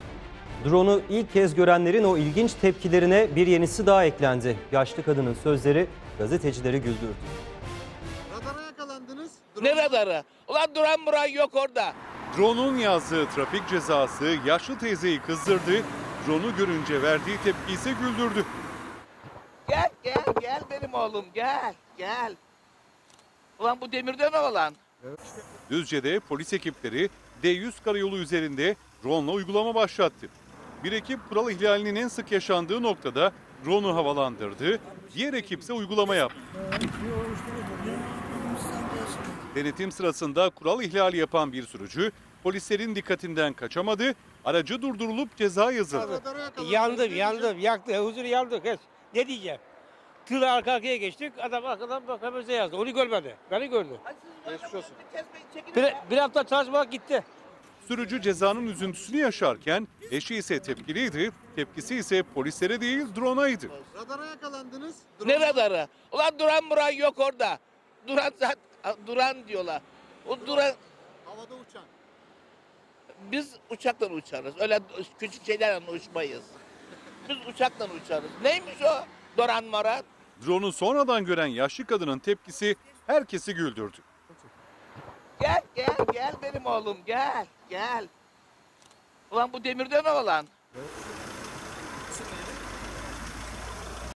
İlk Defa Drone Gören Yaşlı Kadın: "Gel Bakalım Demiroğlan "
Drone'u ilk kez görenlerin ilginç tepkisine bir yenisi daha eklendi. Yaşlı kadının sözleri basın mensuplarını bile güldürdü

TGRT HABER TV HAKKINDA:

● Türkiye'nin kanalıyız...